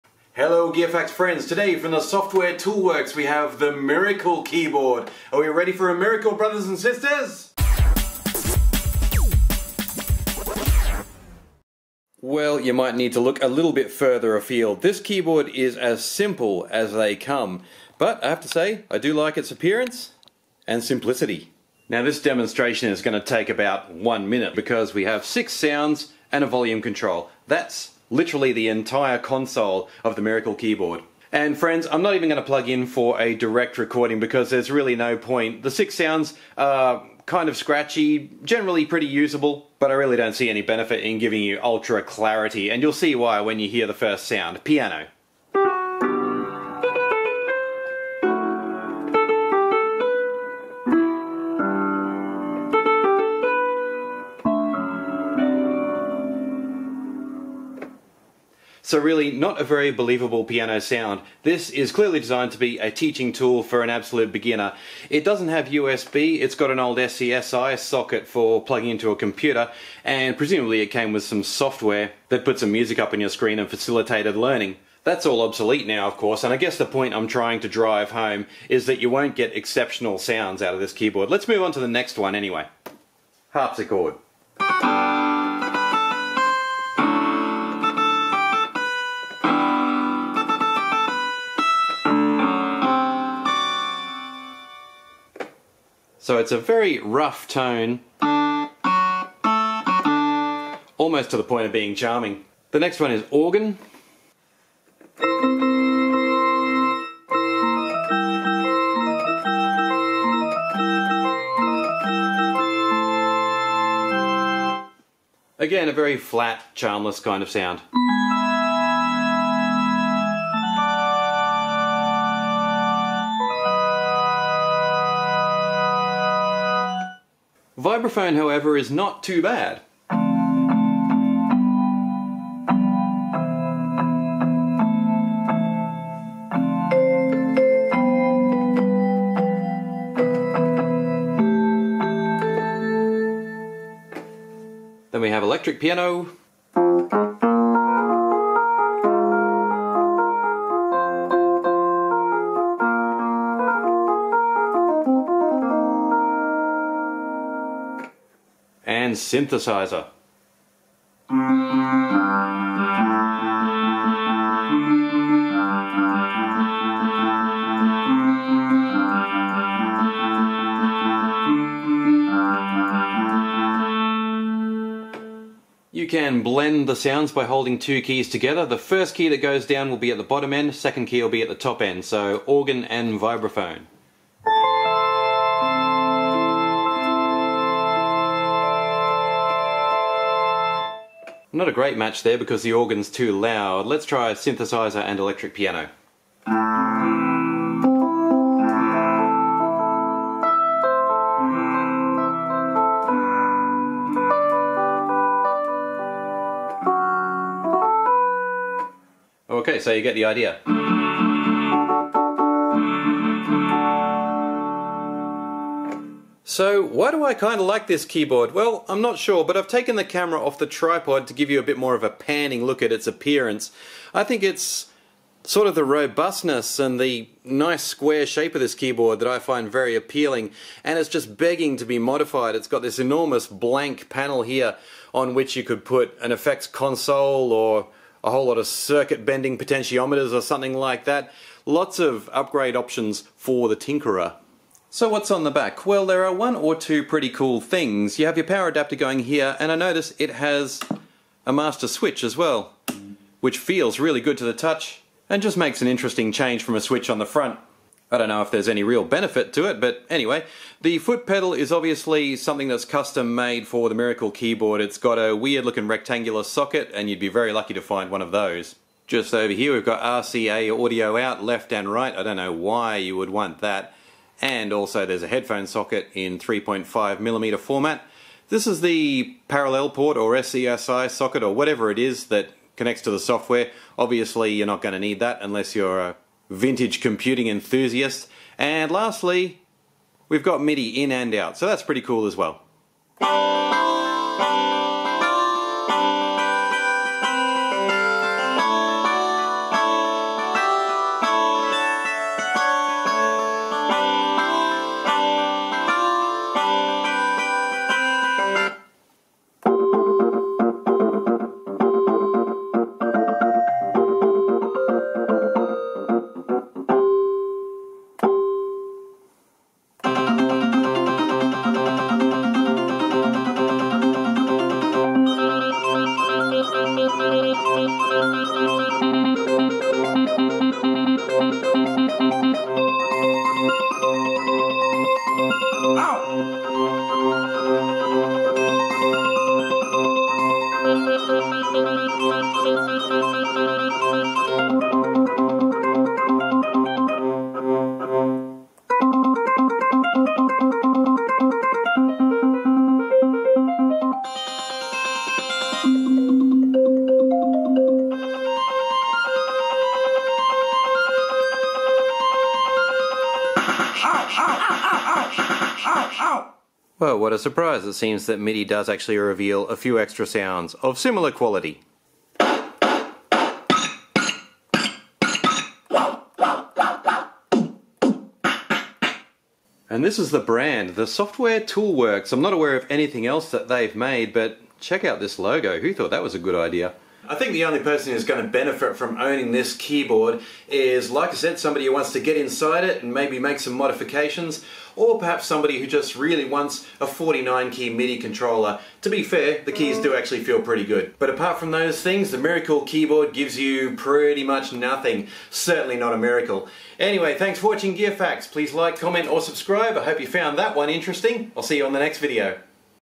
The "Miracle Piano" ...apparently...
Warning: This piano is not a miracle and cannot perform miracles.

It must be one of the first computer-integrated learning systems though.